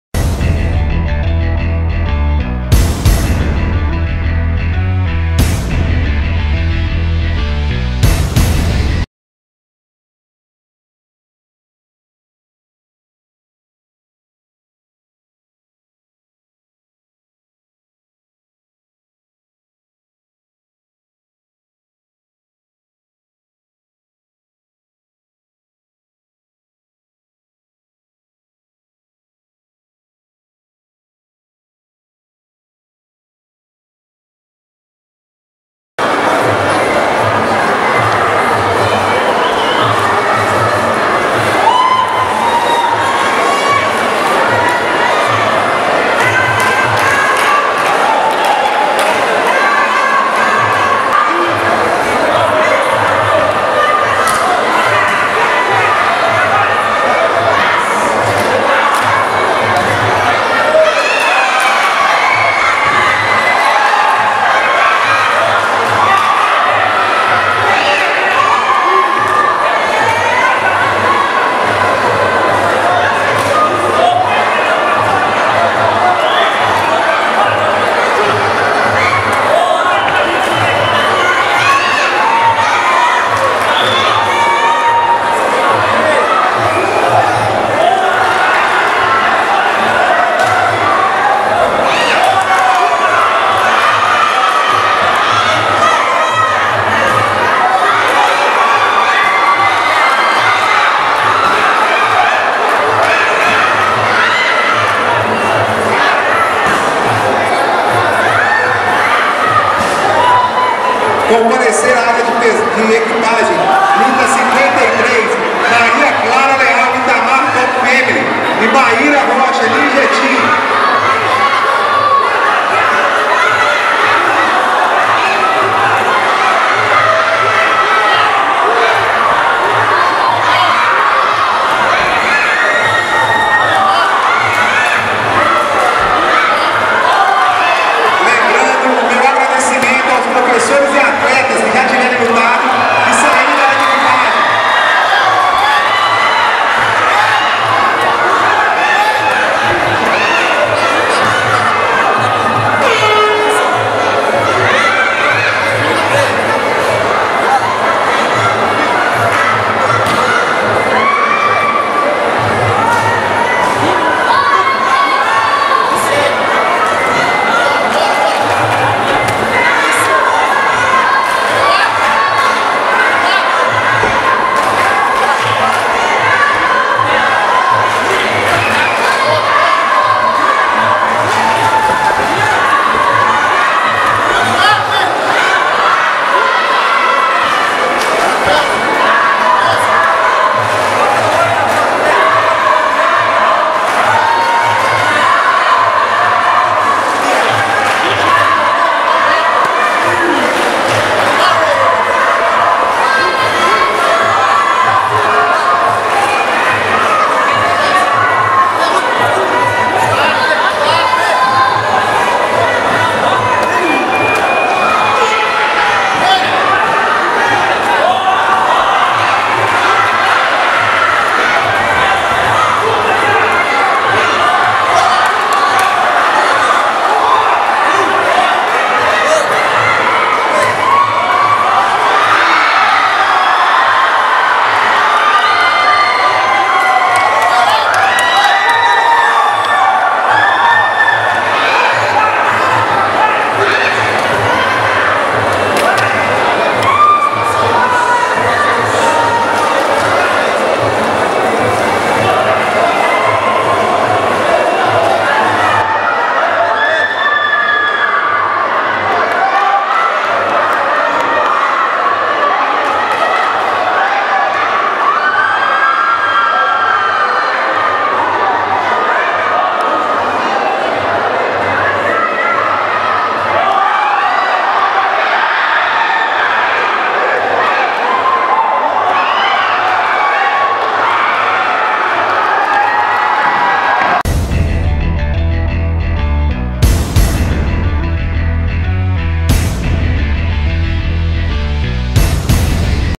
Kelton vs Clécio - Campeonato Mineiro de MuayThai FMTEMG 10ª Edição
Em combate de MuayThai amador até 67kg, Clécio Nascimento (Eliezer Fight Team) venceu Kelton Ferreira (Brazil Wushu Sanda) por nocaute no primeiro round.